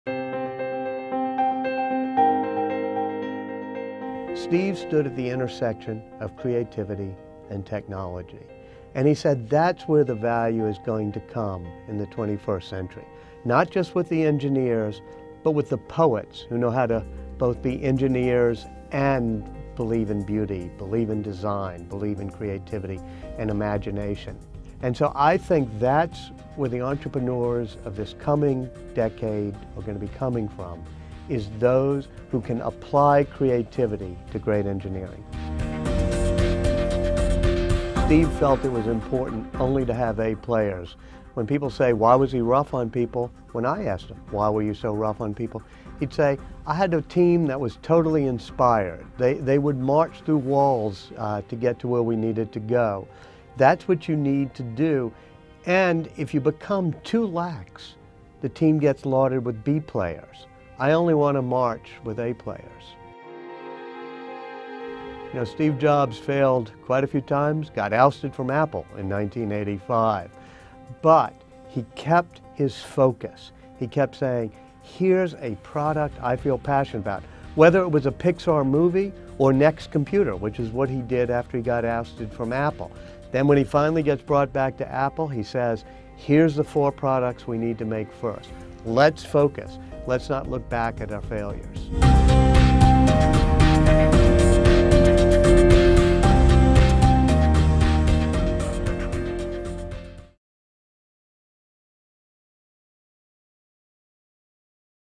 Tips for Successful Entrepreneurship from Steve Jobs
Walter Isaacson discusses Steve Jobs' existence at the intersection of creativity and technology.